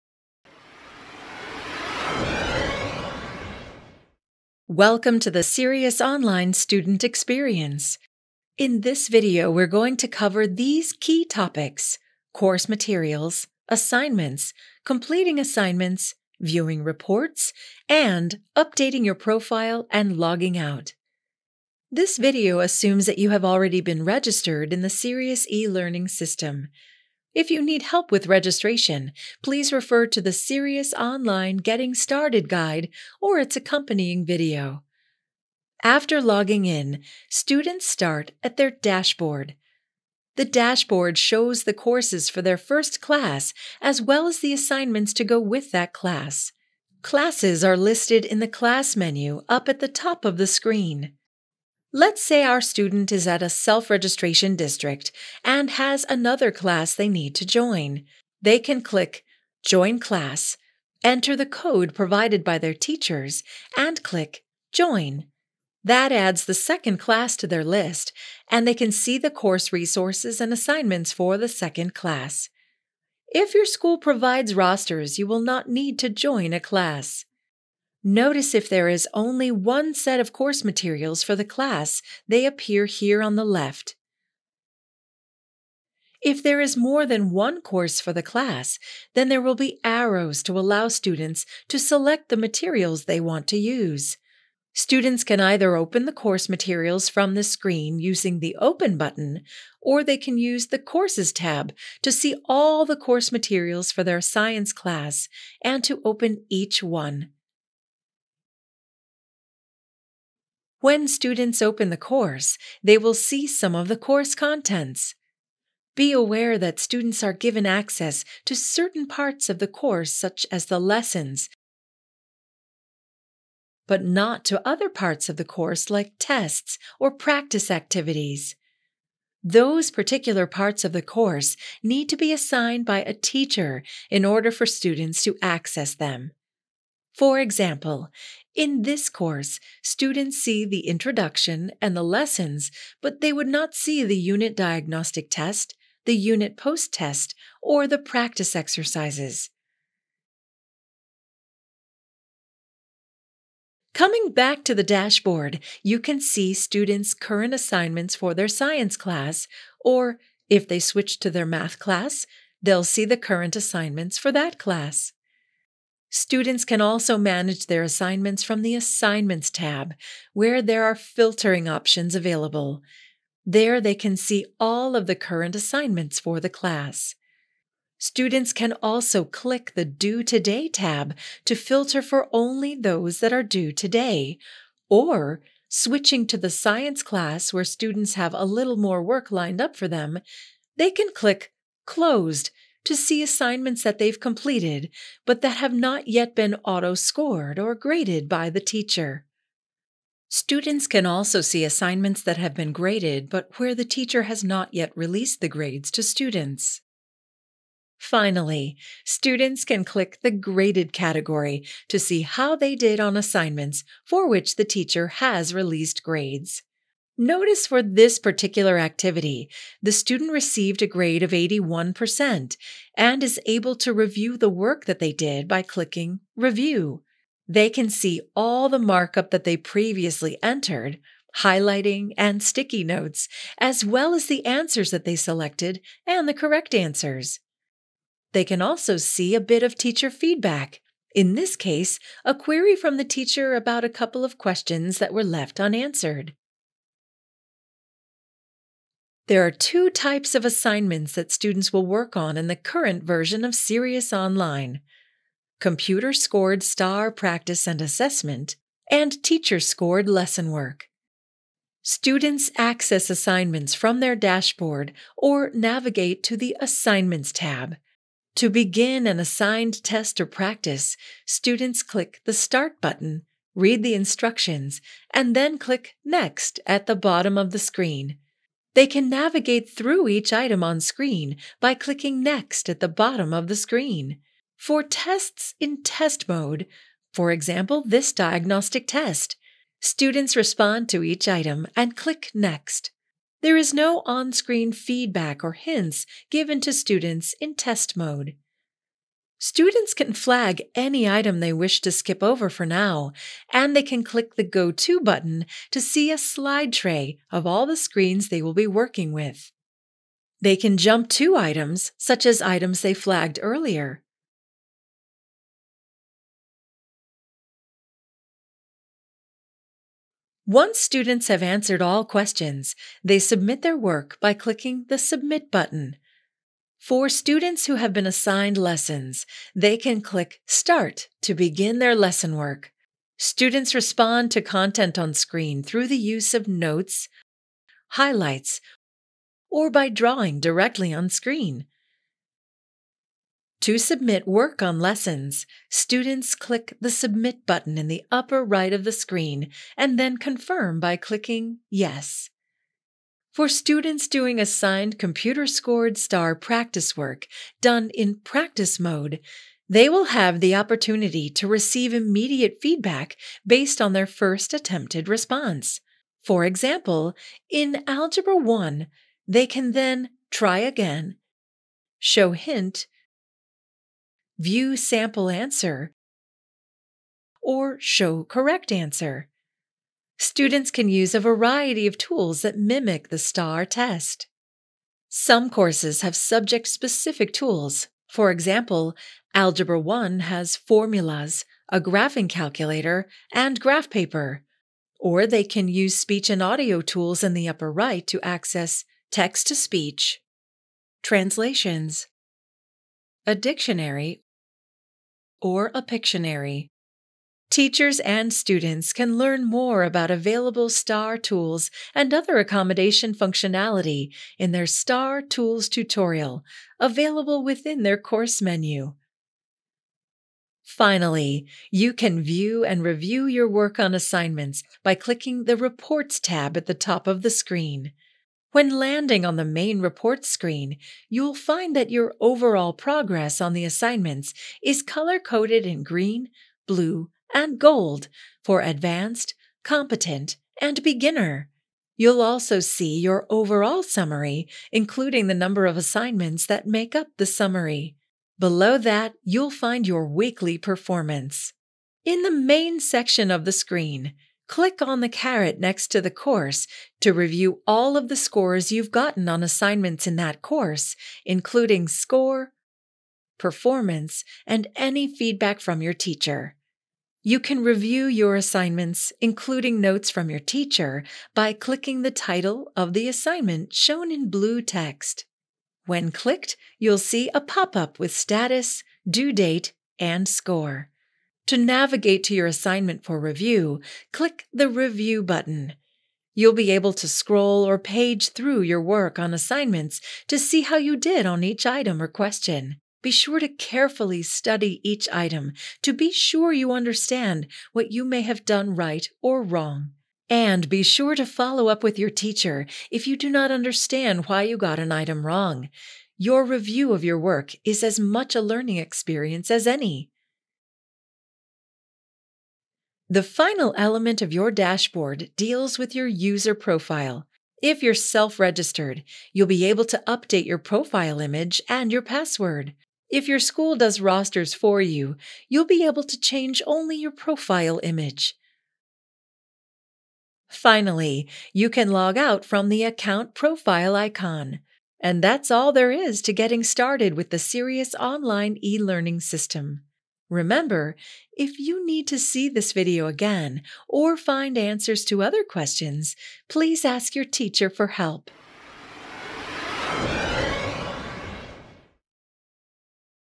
Sirius Online 2.0 Student Experience Guided Tour
0:00 - Introduction/ 1. Getting Started
0:35 - 2. Course Materials
2:33 - 3. Assignments
4:08 - 4. Completing Assignments
7:00 - 5. Viewing Reports
8:32 - 6. Updating Your Profile and Logging Out
8:54 - Conclusion